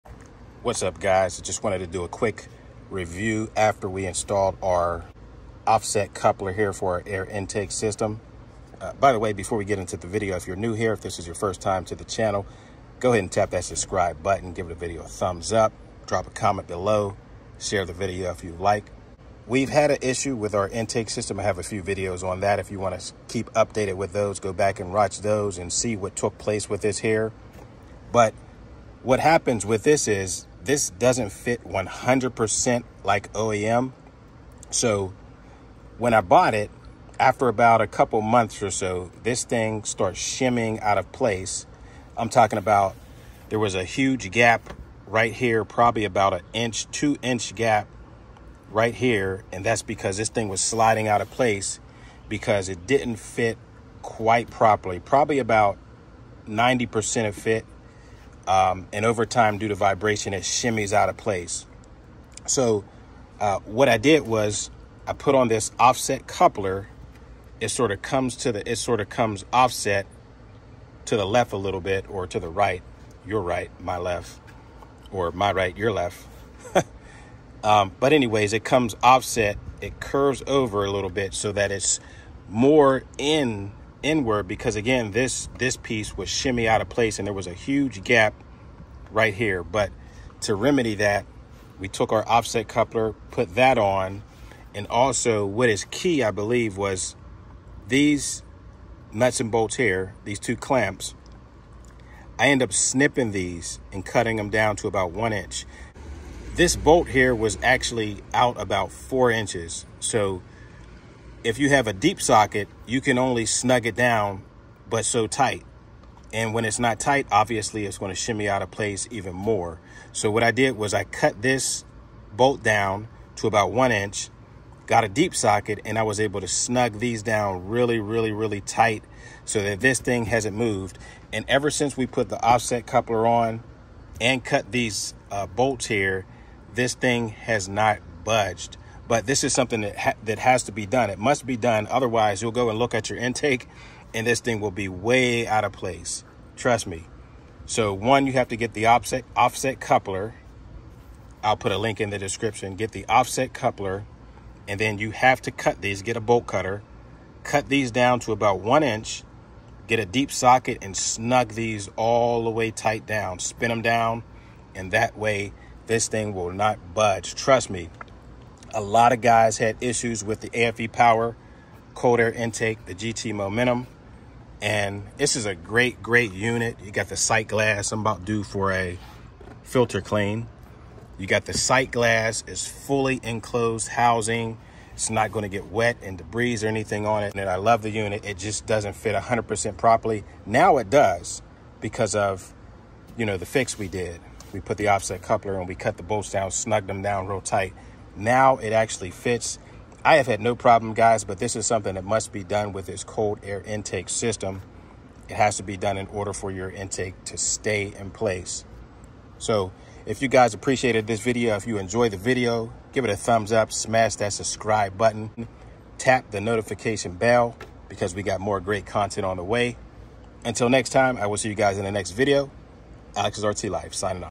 HOW TO PERMANENTLY FIX FITMENT ISSUE ON THE AFE POWER COLD AIR INTAKE!
This must be done to fix this cold air intake system as shown on this video
Purchase the offset coupler below👇🏼